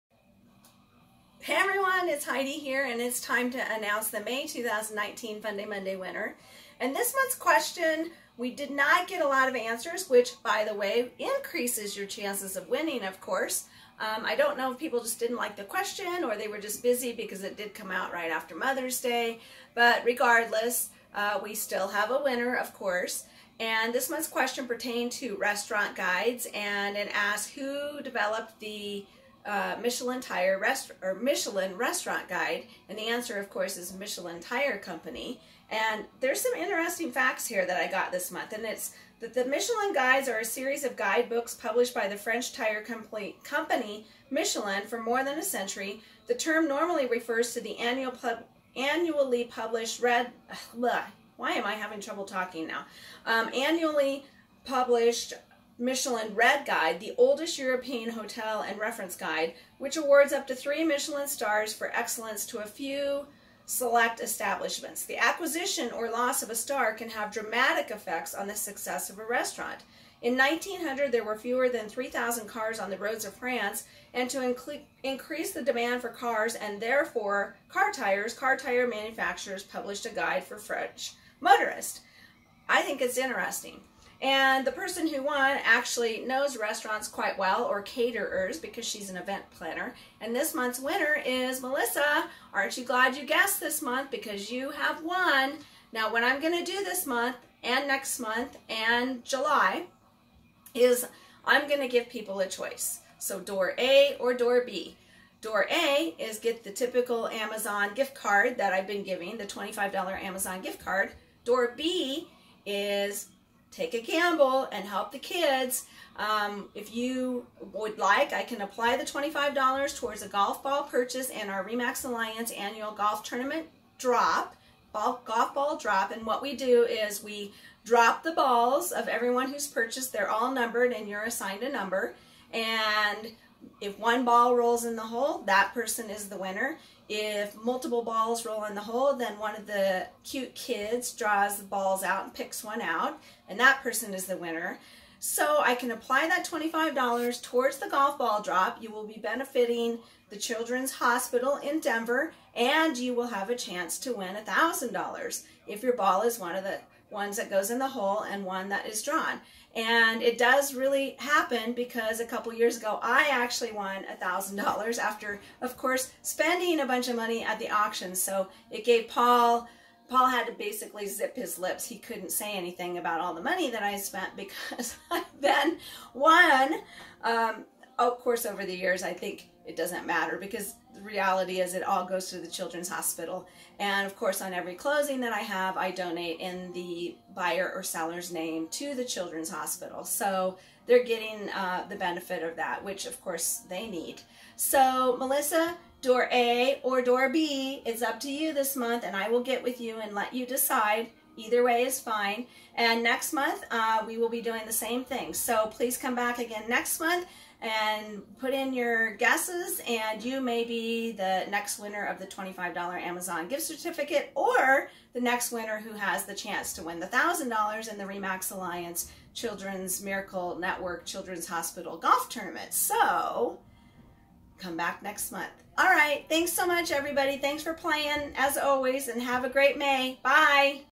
Who invented the Michelin Tire Restaurant Guide? May, 2019, Fun Day Monday Question
This is the announcement of the answer and the winner this month